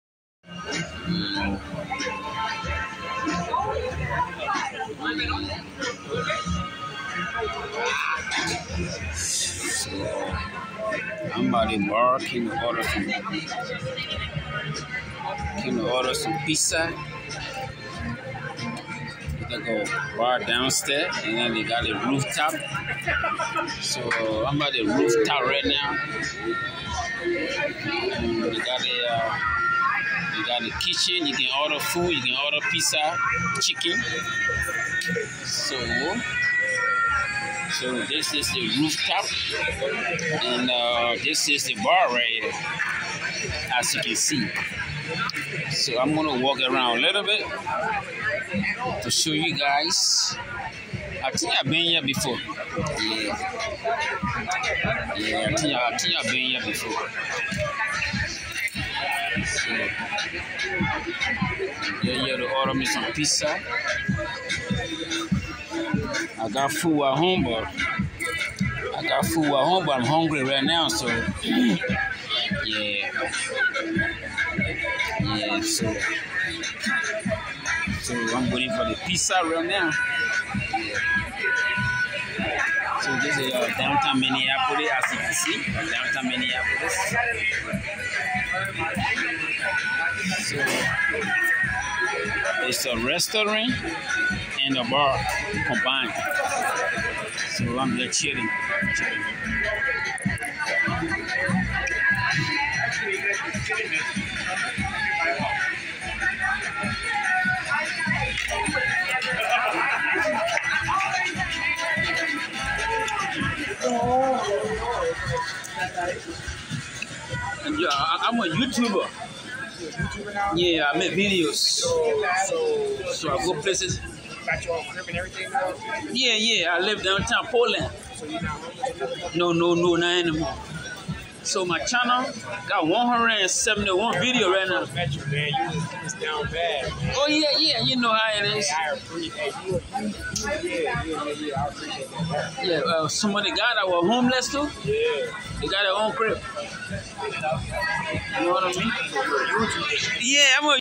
Downtown Minneapolis Ordering Pizza
I was downtown Minneapolis taking a scroll and decided to go to the roof top to order some pizzas but he gave me hot wings and spicy honey sauce.